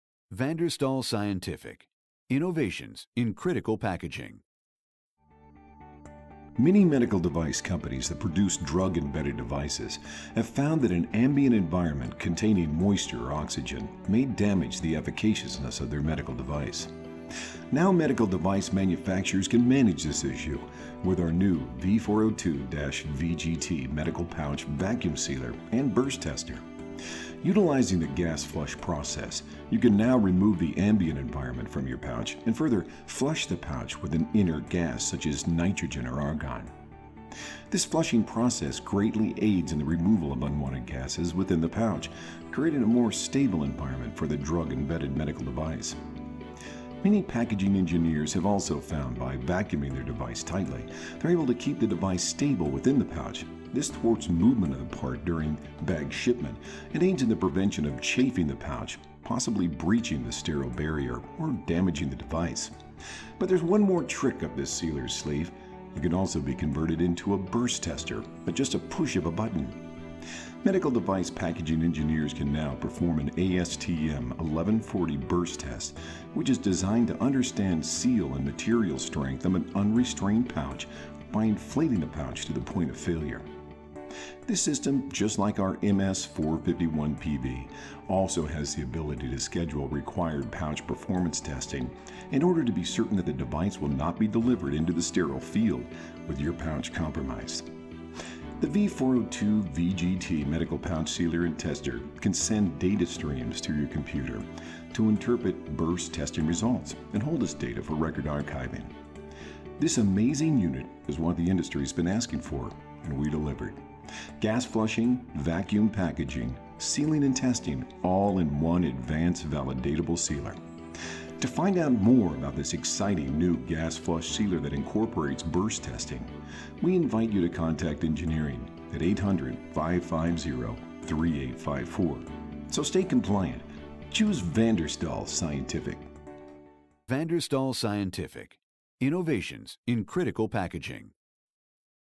Medical Pouch Vacuum Sealer and Burst Tester - Van der Stahl V-402-VGT
Van der Stahl V402-VGTs patent pending technology is slated to change the way medical device packaging companies package atmosphere critical medical devices. Many medical device companies that produce medicine embedded devices have found that an ambient environment can produce moisture within the pouch have damaged the efficaciousness of their embedded drug.